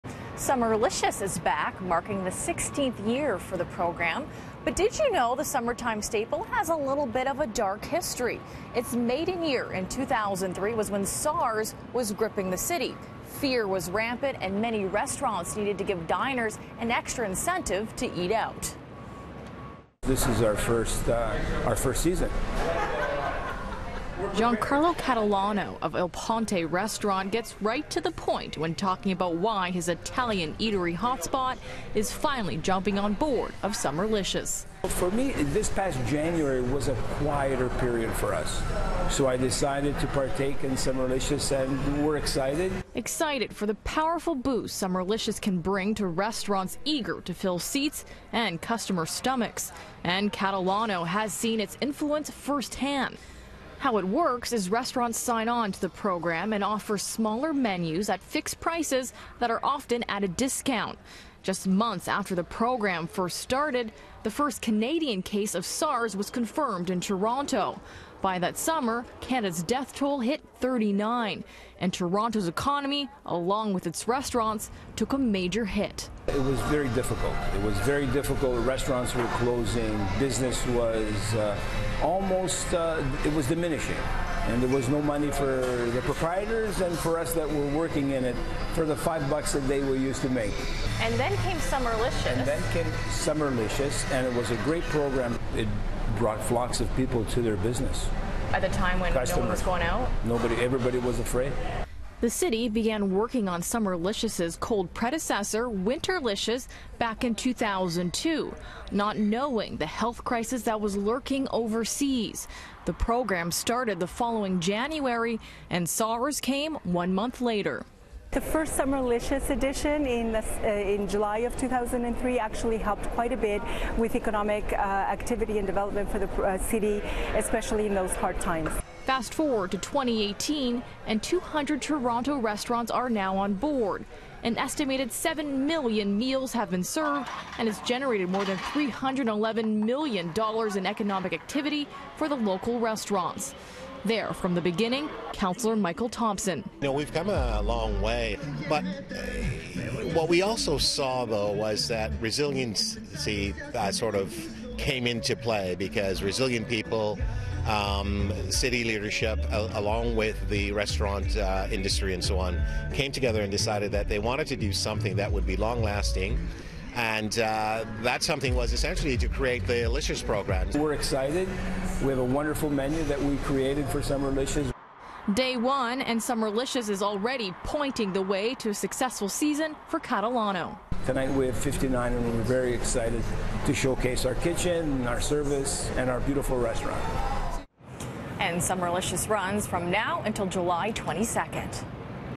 Summerlicious and the dark history behind it
Summerlicious is marking it's 16th year in Toronto but did you know the summertime staple has a dark history? It started as a way for restaurants to give diners an extra incentive to eat out during the SARS outbreak